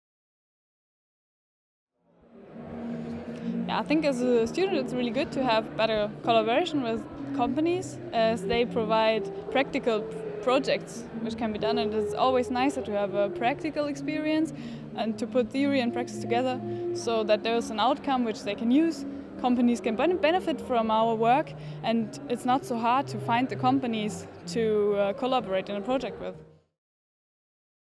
Interview - Wiebke Tschorn, Master student
Interview with Master student in Business Relationship Management programme, Wiebke Tschorn, about the Danfoss Center of Global Business.

Opening of Center, 29th of January 2014, Alsion